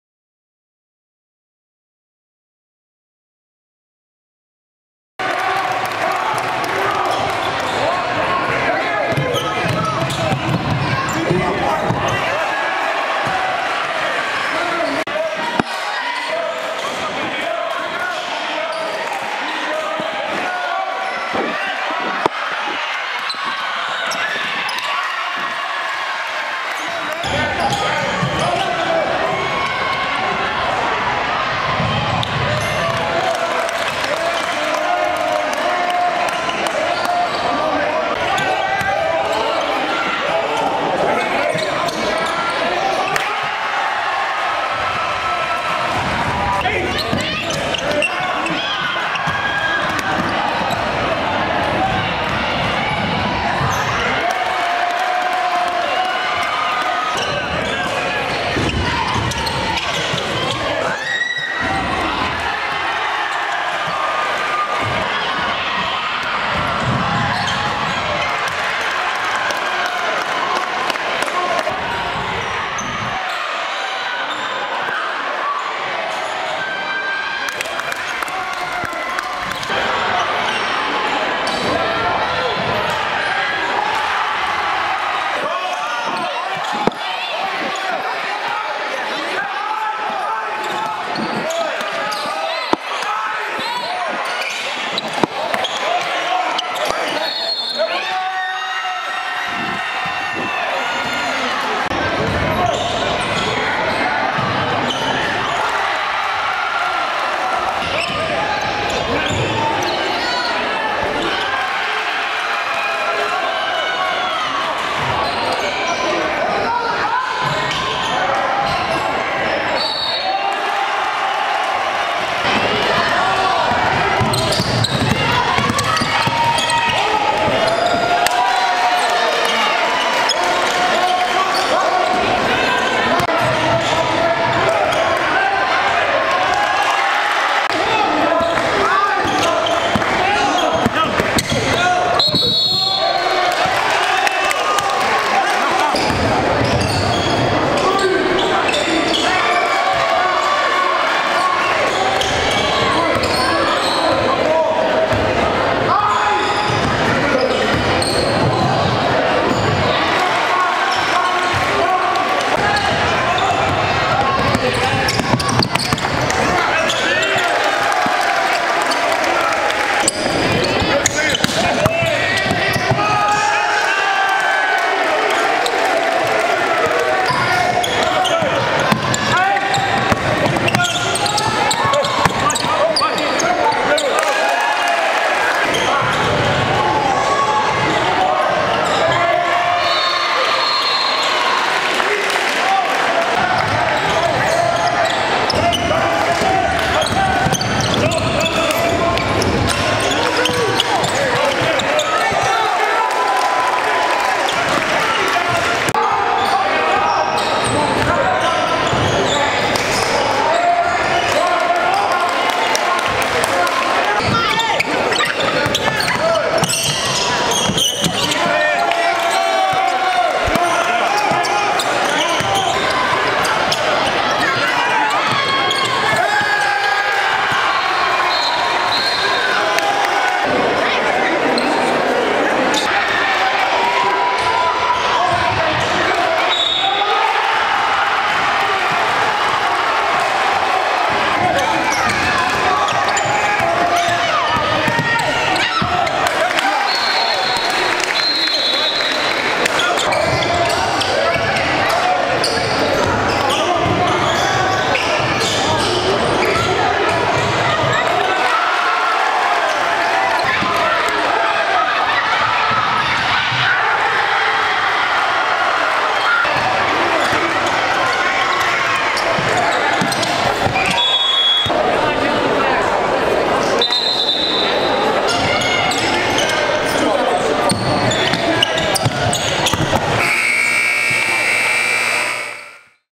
TUSCALOOSA COUNTY AND BRYANT GO DOWN TO THE WIRE!
In this Tuscaloosa area showdown, Bryant High School is narrowly defeated by Tuscaloosa County.